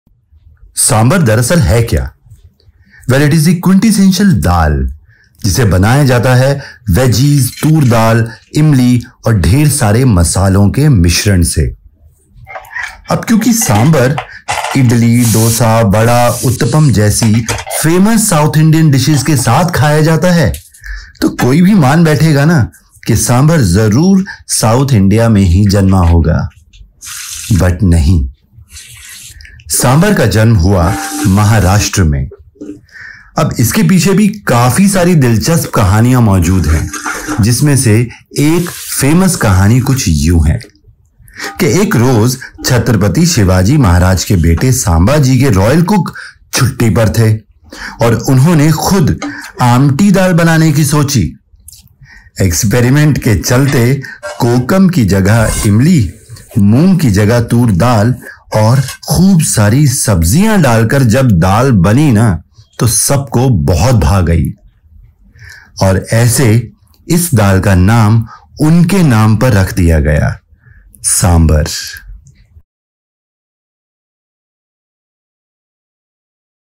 SAMBHAR QUICK RECIPE | सांभर साउथ से नहीं आया तो कहाँ से आया ? 🙄🤷🏻‍♂️
SAMBHAR | सांभर
All you need 👉
To boil Daal :
Toor Daal 1 1/2 cup
Water 4 to 5 cups
Tadka :
Oil 2 to 3 Tbsp
Mustard Seeds ( Raai ) 1 tsp
Cumin seeds 1 tsp
Fenugreek seeds ( Methi Dana ) 1/4 tsp
Whole red chillies 2
Ginger and green chillies crushed 1 Tbsp
Heeng 1/2 tsp
Madras onions as per liking
Sambhar Powder 3 Tbsp full or as required
Veggies ( carrots , drumsticks , beans , bottle guard ( ghiya ) , pumpkin ( kaddu ) , tomatoes )
Water 4 cups or as per liking
Tamarind water 1/3 cup
Jaggery grated 2 Tbsp
Serve Hot Sambhar with Idli , Dosa , Vada , Uttapam and other South Indian Dishes. Enjoy 😍